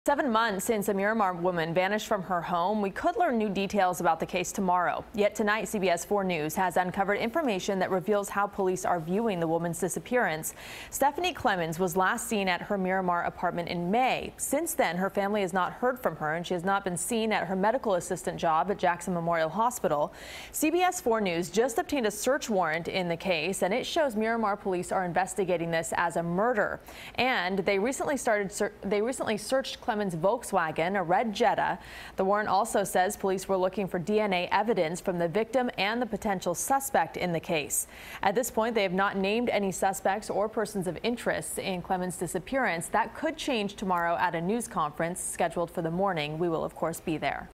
Police Have New Details On Miramar Woman Missing Since May
The case of a missing Miramar woman is now considered a murder investigation.